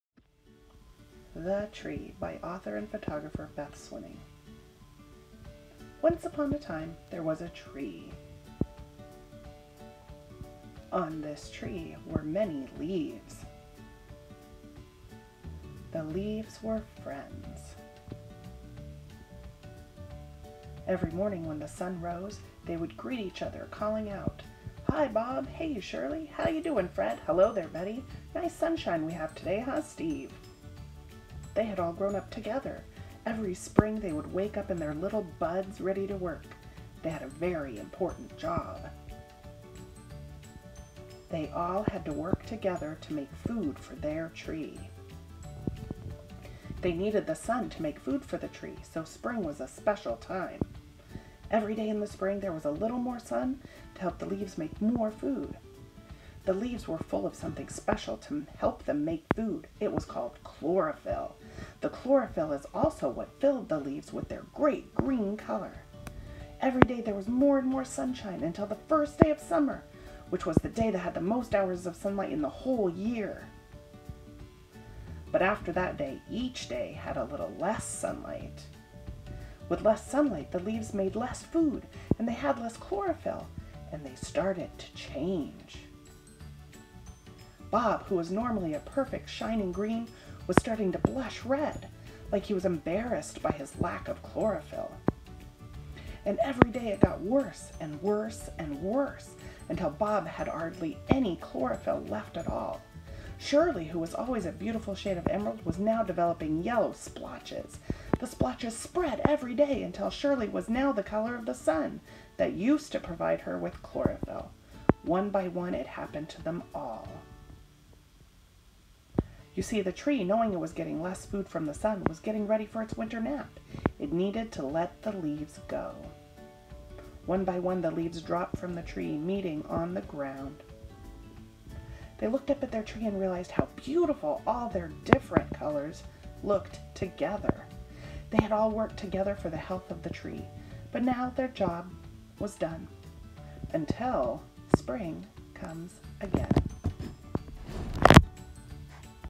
The Tree
A photo narrative for young children about leaves changing color in the Fall.